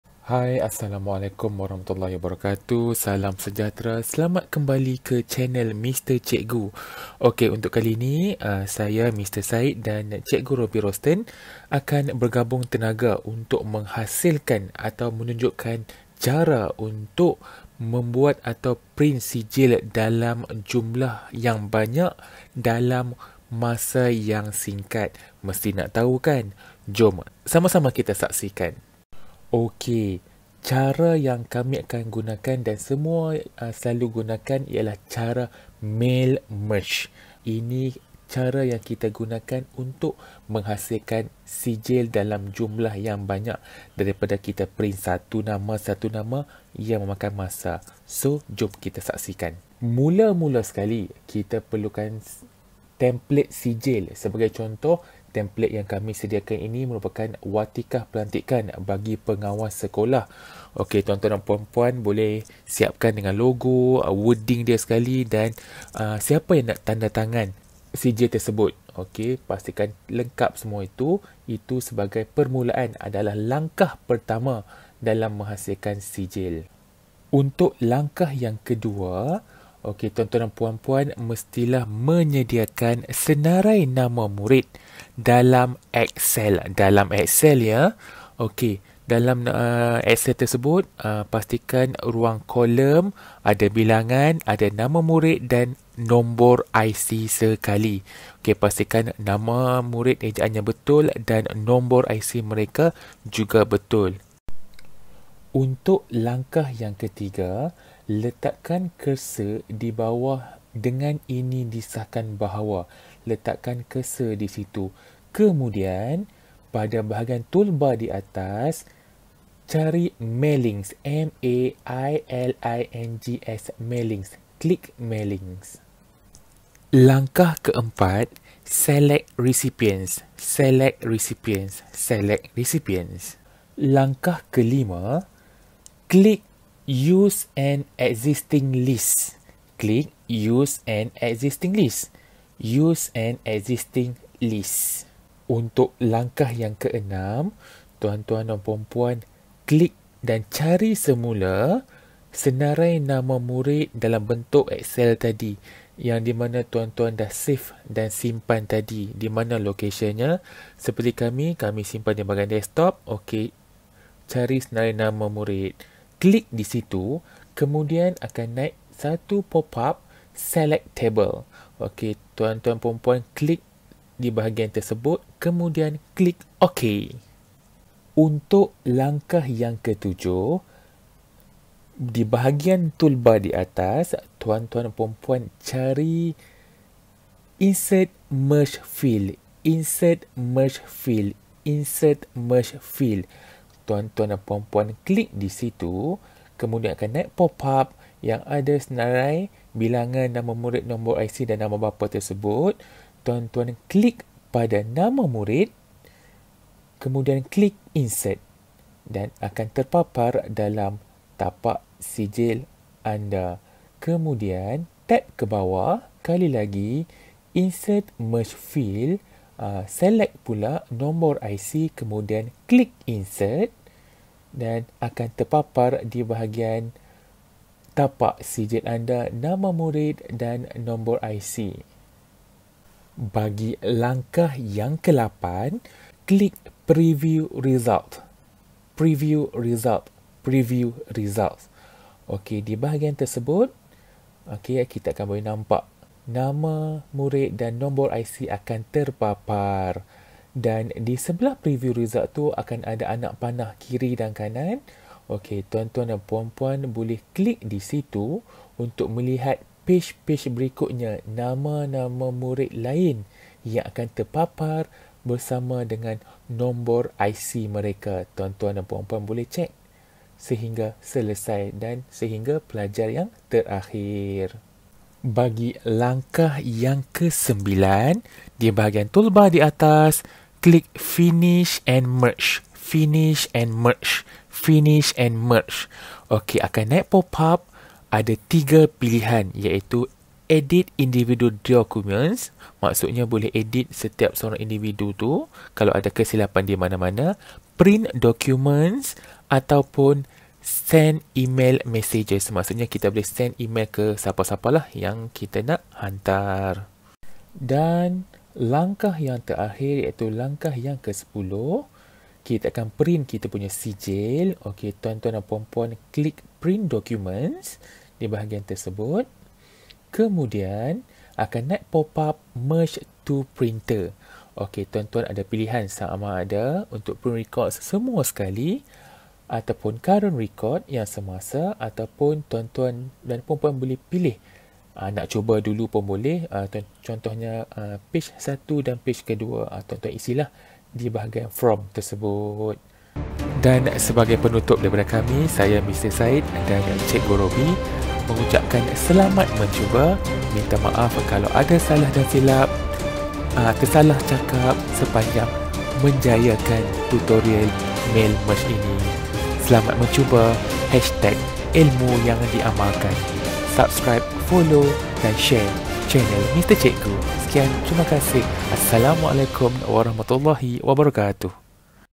Cetak sijil yang banyak dengan cepat
mencetak sijil yang banyak akan mengambil masa yang lama untuk menyiapkannya..Namun, dengan menggunakan kaedah Mail Merge, kerja kita akan menjadi sangat2 cepat..jom pelajari dan amalkan..